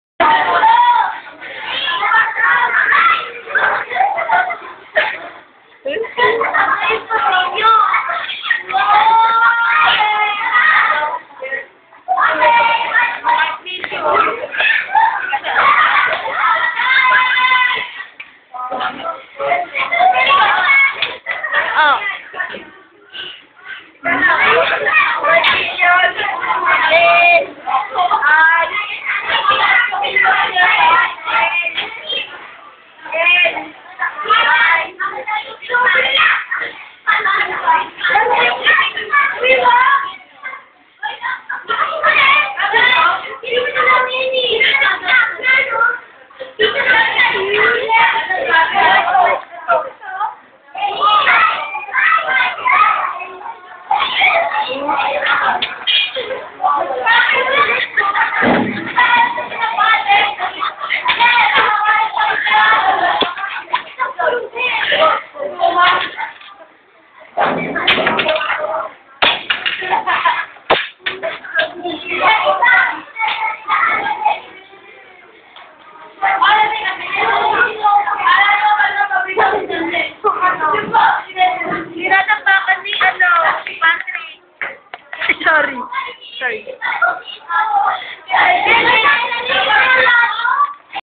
scandal of 6 moses in jcklfs
ito na po ung first scandal ng 6 moses during thier lunch time